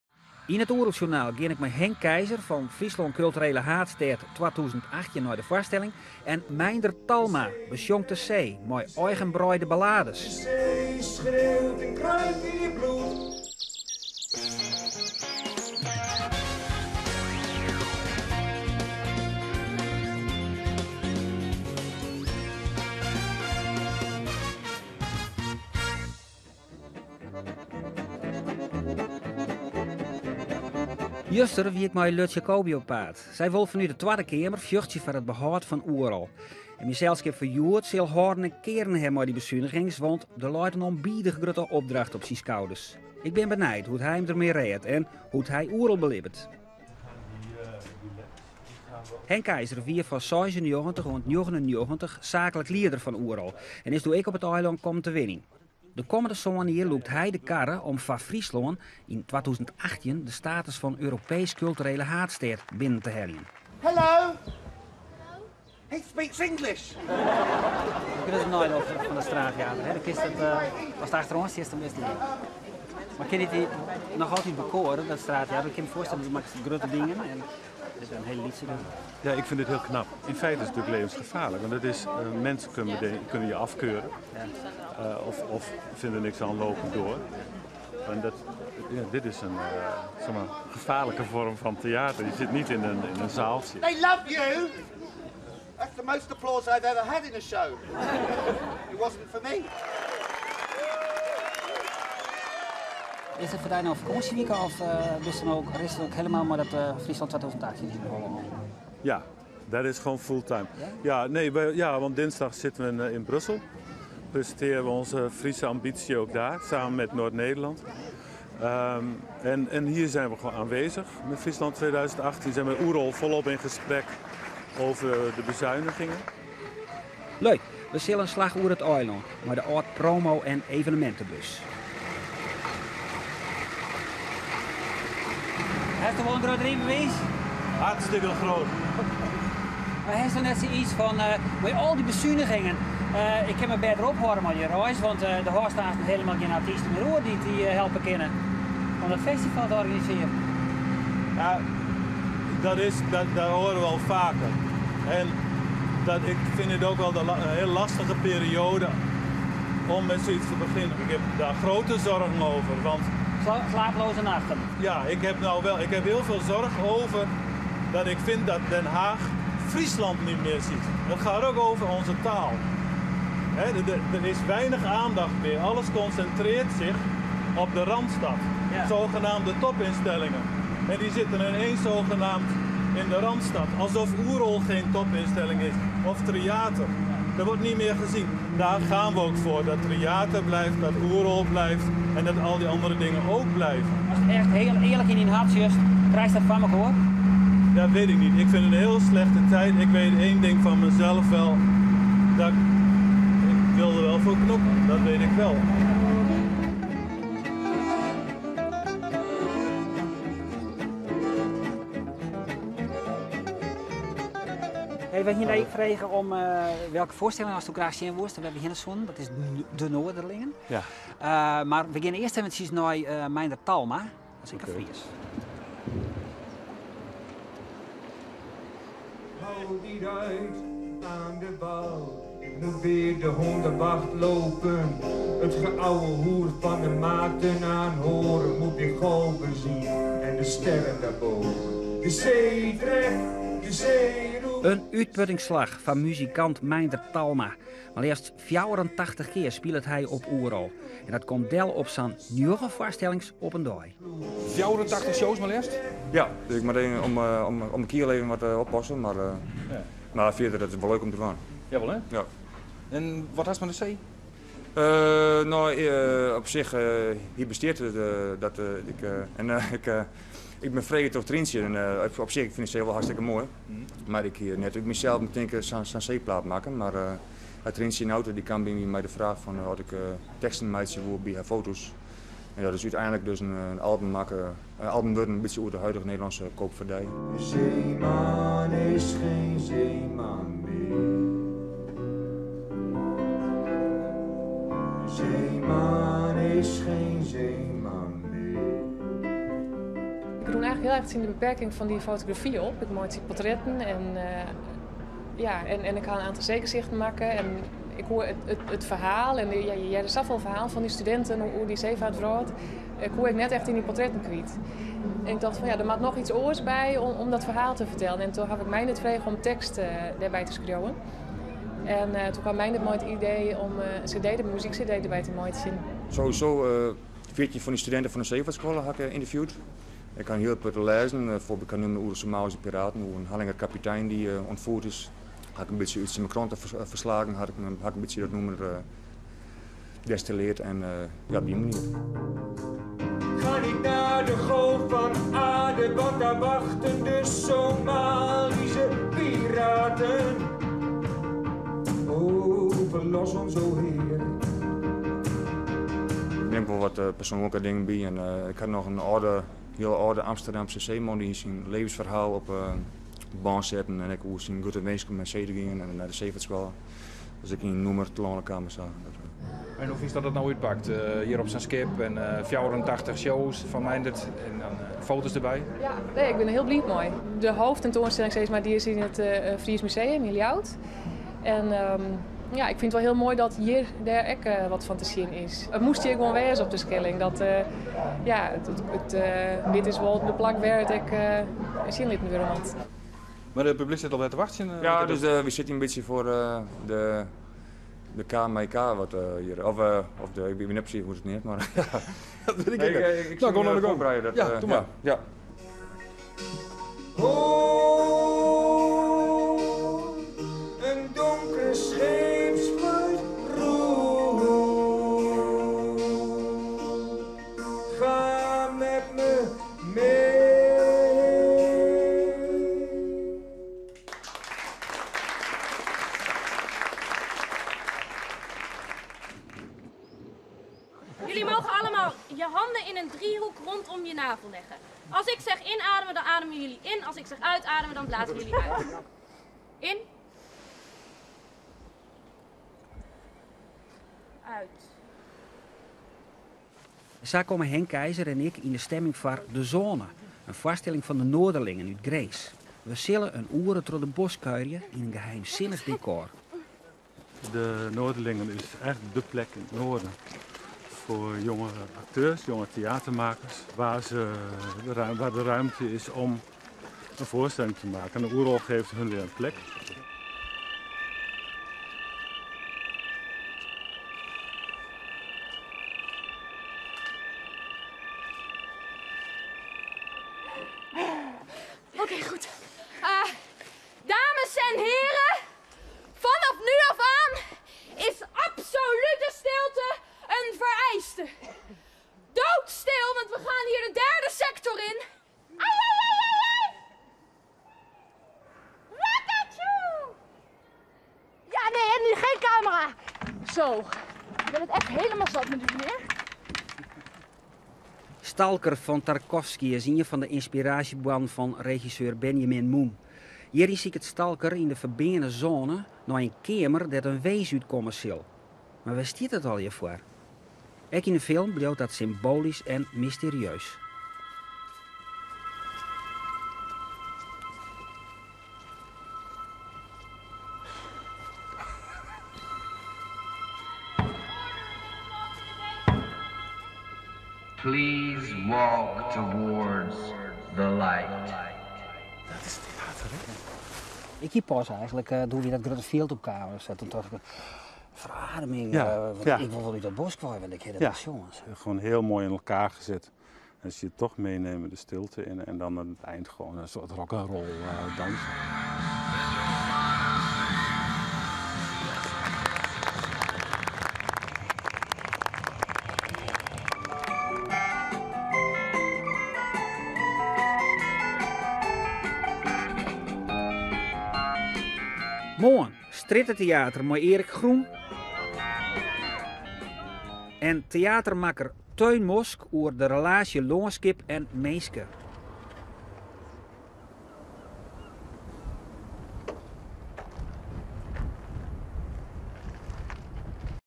Oerolsjoernaal tiisdei 21 juny 2011
Hjoed giet Albert Jensma op in paad mei de kertiermakker fan Fryslân Kulterele Haadstêd 2018 Henk Keizer.
Henk Keizer wie fan 1997 oant en mei 1999 saaklik lieder fan Oerol. Hy wennet sûnt dy tiid op Skylge. We begjinne mei in ritsje yn 'e promoasje bus fan Fryslân 2018. Troch alle besunigings yn 'e kultuersektor is it foar Fryslân 2018 ek hieltyd dreger om it eindoel te heljen: Ljouwert Kulturele haadstêd fan Europa yn 2018. Keizer leit der wolris wekker fan!